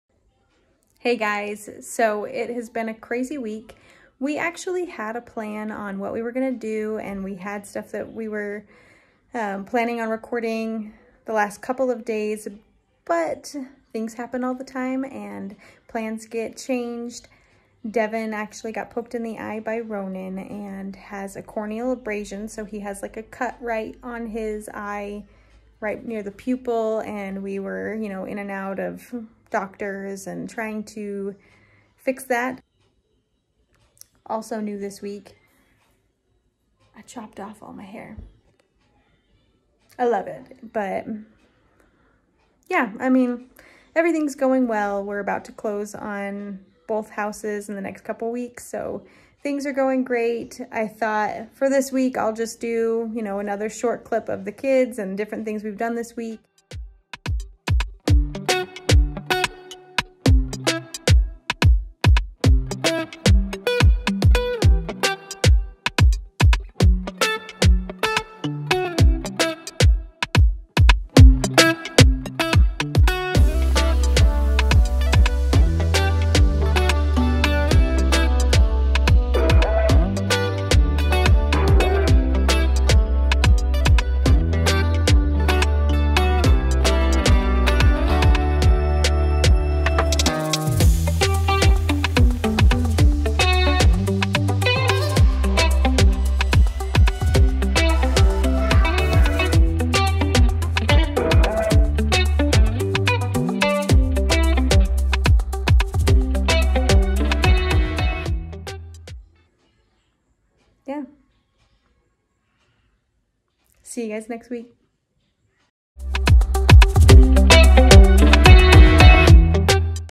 Poked Eyes, Haircuts, Kids, and Failed Plans.
This week we had a plan, but then Devan got poked in the eye while playing with Ronan, and that plan failed.. Oh well, things happen! I did get a haircut this week, we had a fun double date night and the kids had a great time, as usual.